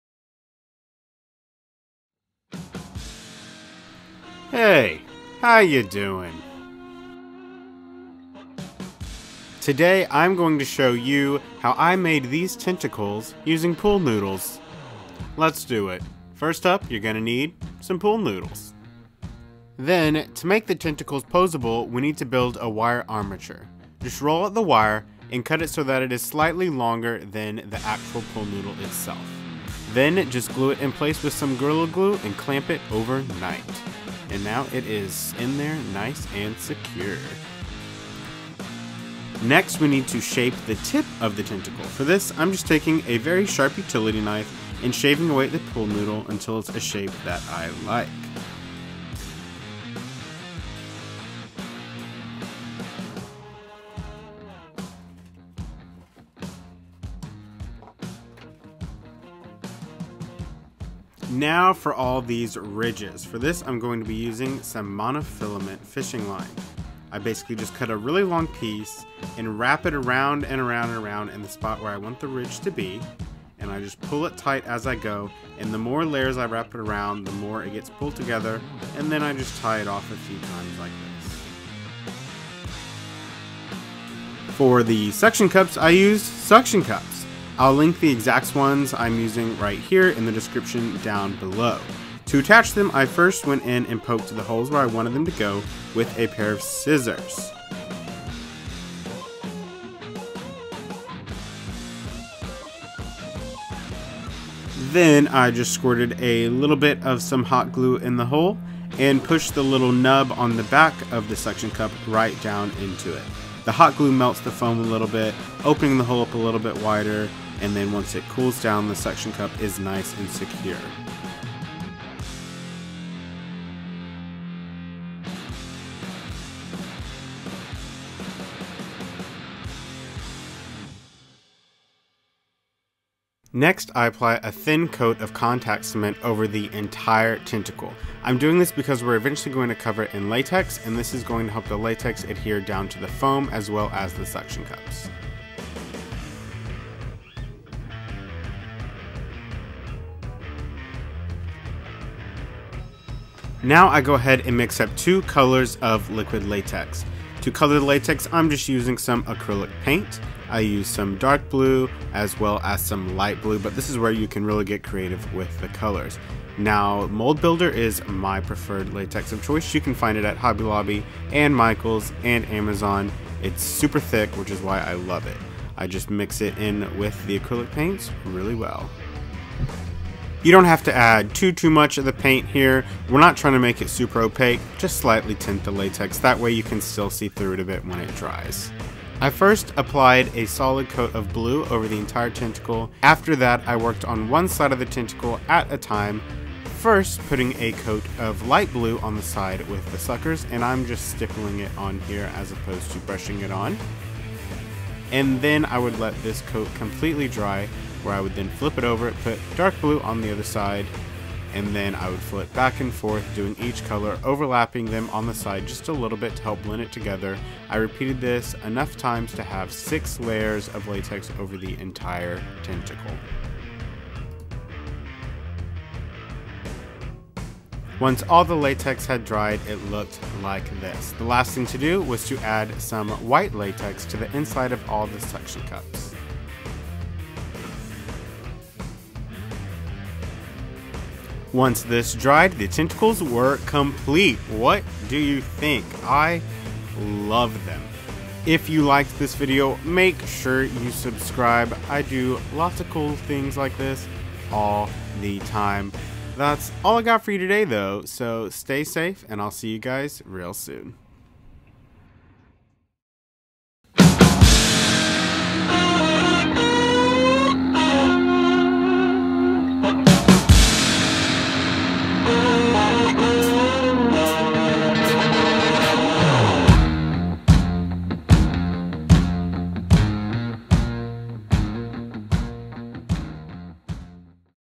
How To Make Tentacles - Halloween Prop Tutorial
Today we make tentacles using pool noodles and liquid latex.